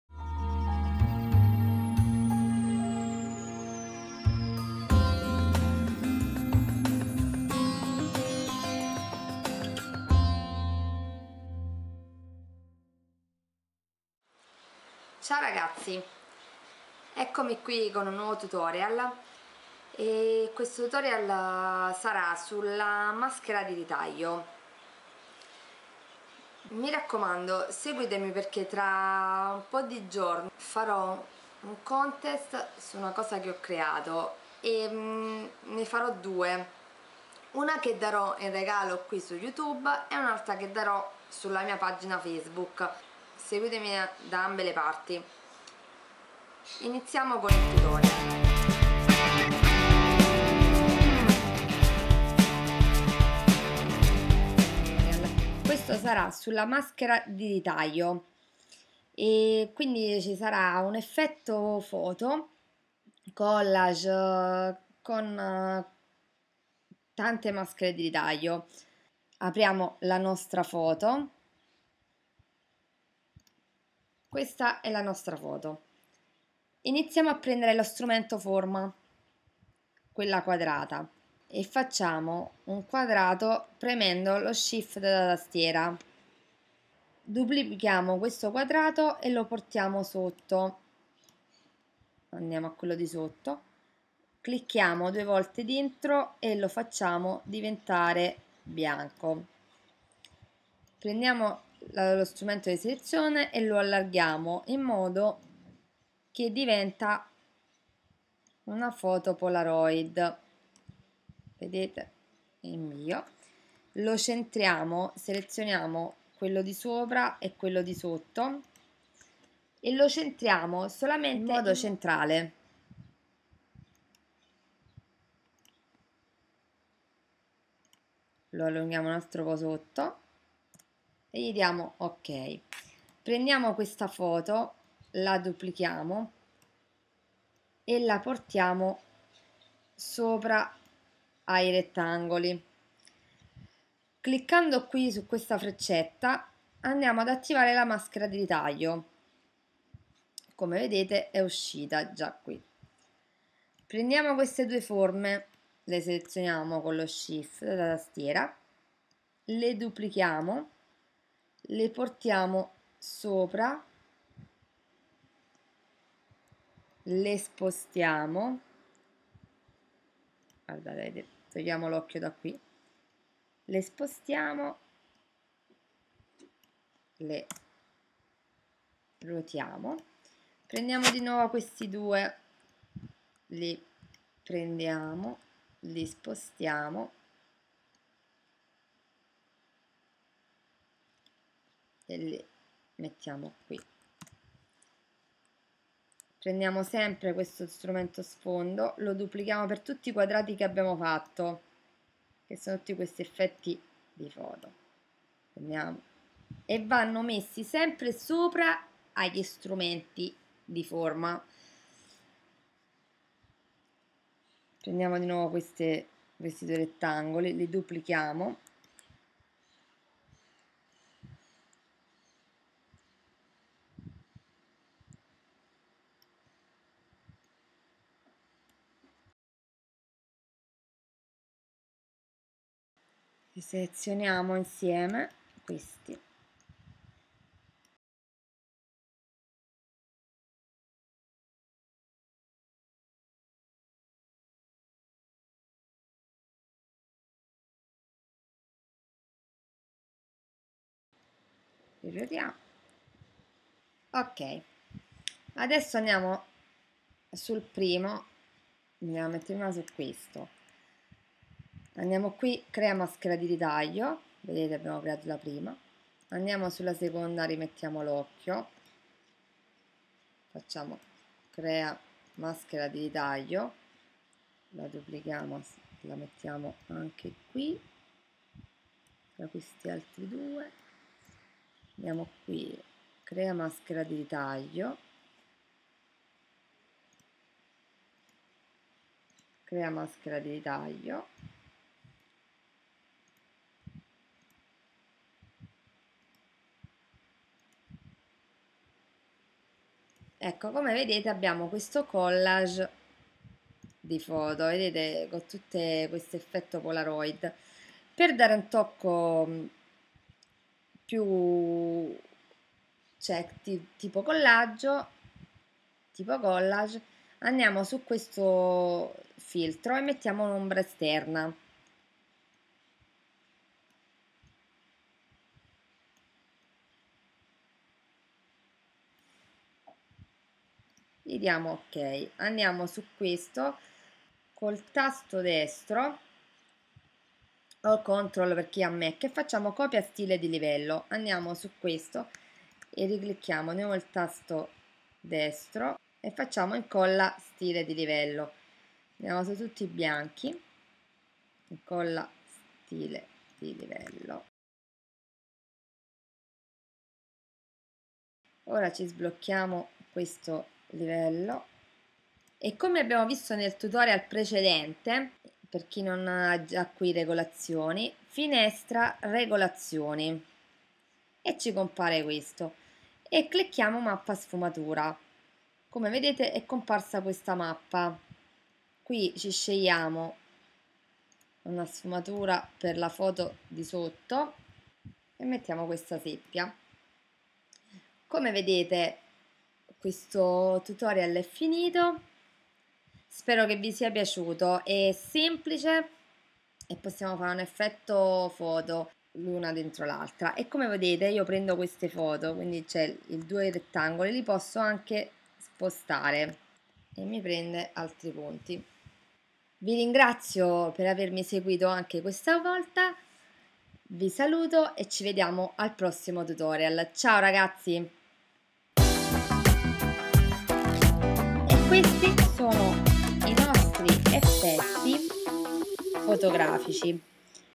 COLLAGE CON EFFETTO POLAROID - Photoshop tut.49
In questo tutorial vediamo come creare attraverso lo strumento forme , tanti scatti a forma di foto polaroid, per ottenere un effetto collage.

Grazie